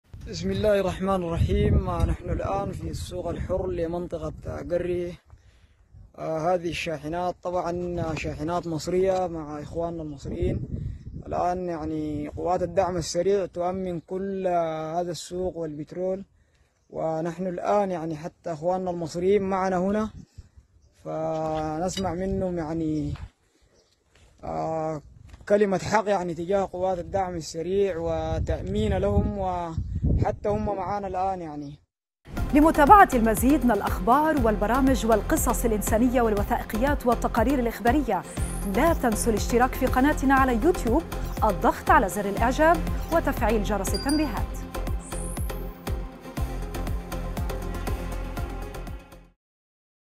السودان.. الدعم السريع ينشر فيديو يقول إنه لتأمين سائقي الشاحنات المصرية بمنطقة قري الحرة
الدعم السريع ينشر فيديو يقول إنه لتأمين سائقي الشاحنات المصرية بمنطقة قري الحرة